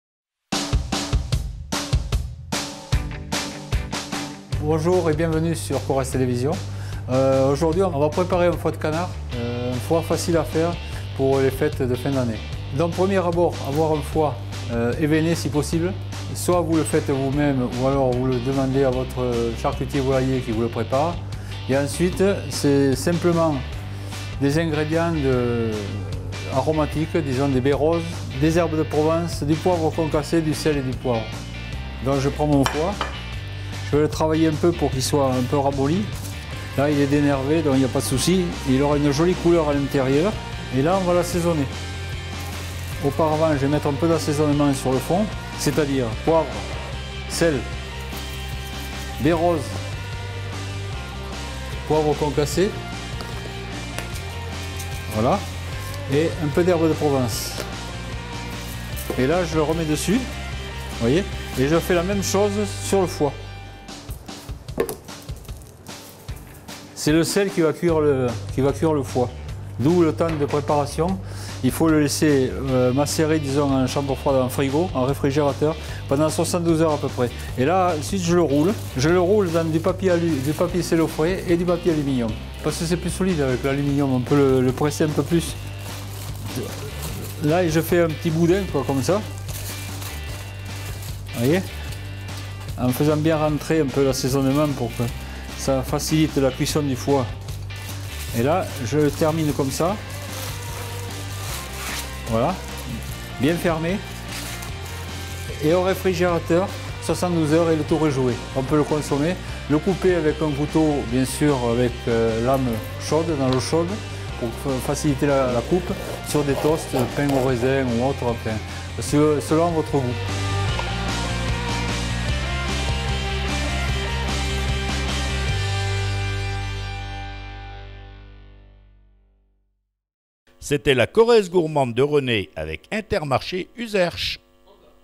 La Corrèze Gourmande de René : Spéciale Foie Gras
Toujours en recherche d'idées pour les fêtes ?
René vous propose une recette rapide pour une entrée de foie gras !